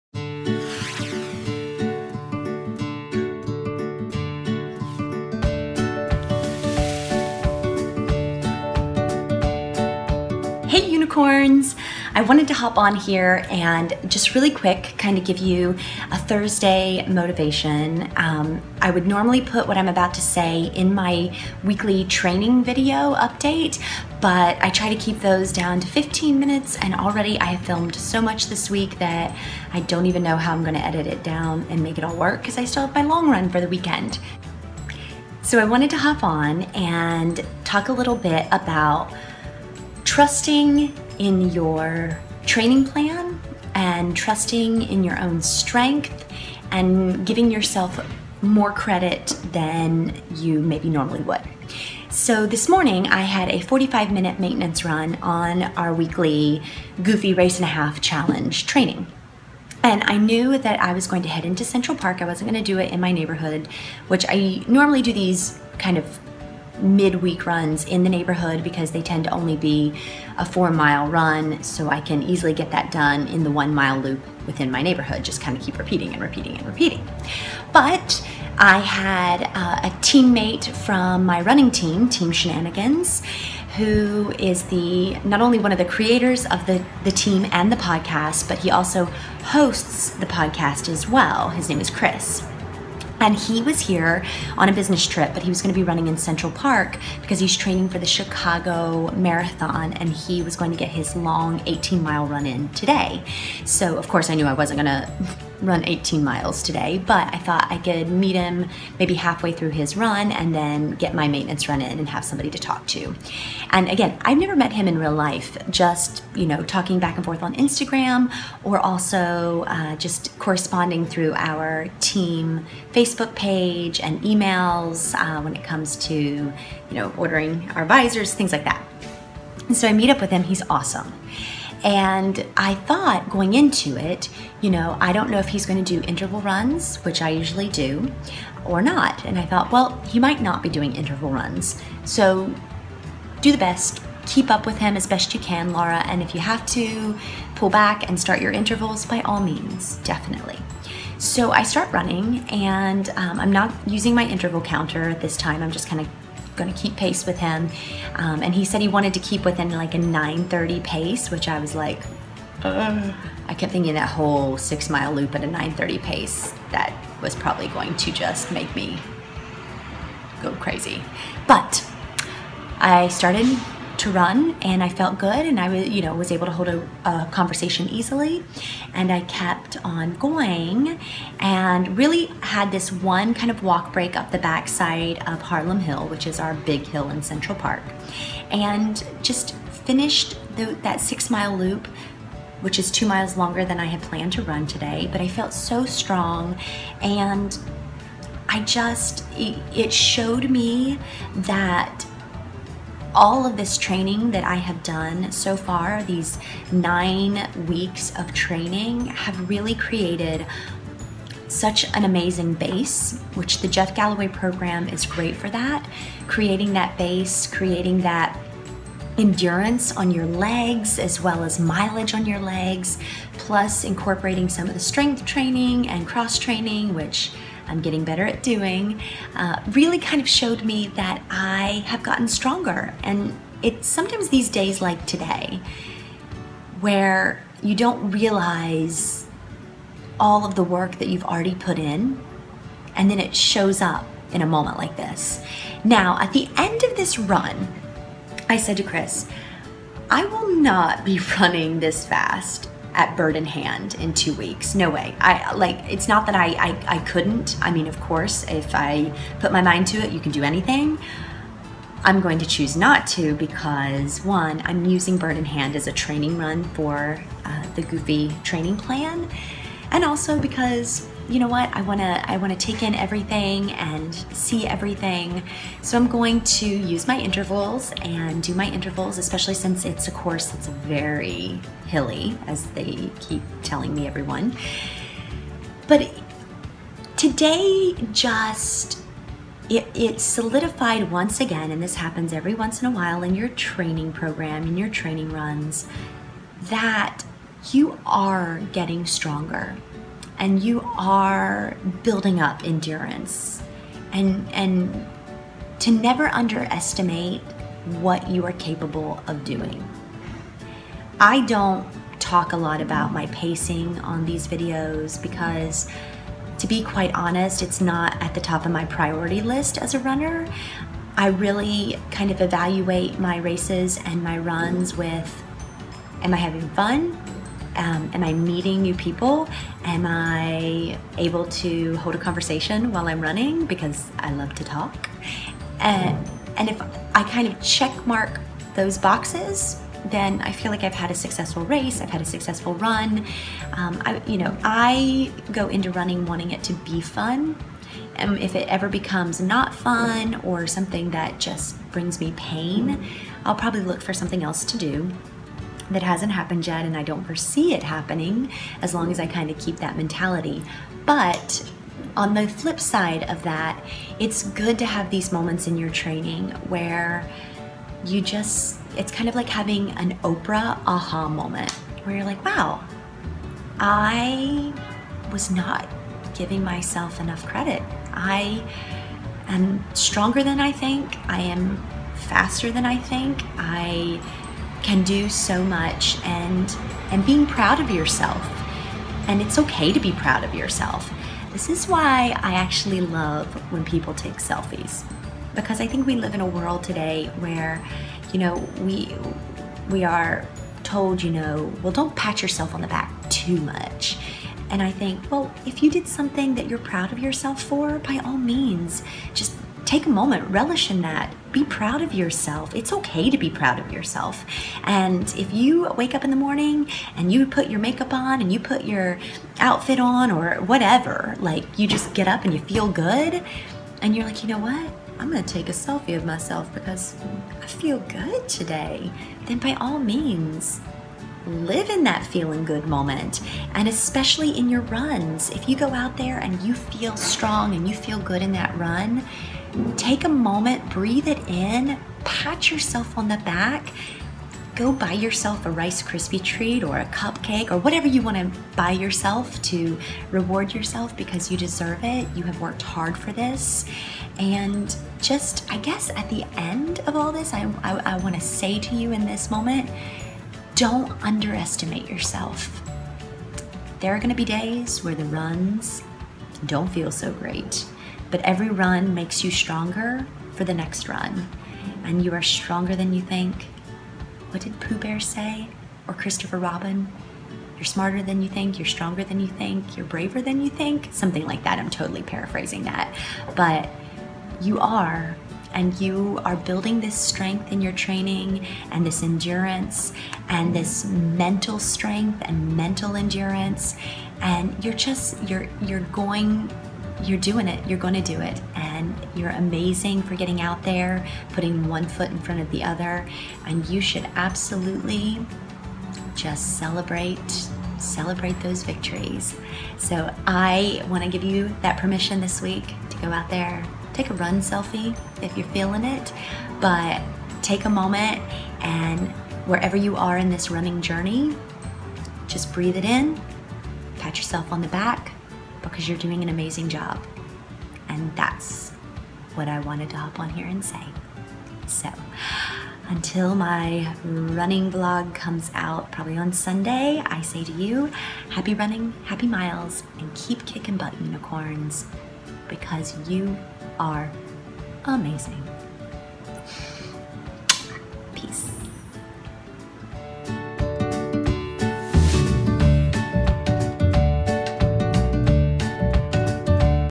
End of Week Motivation: PRAISE BREAK!! YOU ARE STRONGER THAN YOU THINK!!
Hey you amazing Unicorns! I just got back from one of those runs that reminded me that this training is making me stronger and I just had to jump on and remind you, that you are also STRONGER THAN YOU THINK!!

Sometimes we need those days in our training, that show up when we least expect it, to give us a glimpse of our hard work and where it has brought us.

This coming weekend and next week, give yourself permission to relish in the good moments. Pat yourself on the back, buy a cupcake, take an awesome run selfie, or whatever makes you happy, but give yourself that reward, because you are KICKING BUTT and you should be so proud of yourself!

Happy Running, Happy Miles, and Keep Kicking Butt Unicorns!!!
xoxo
Laura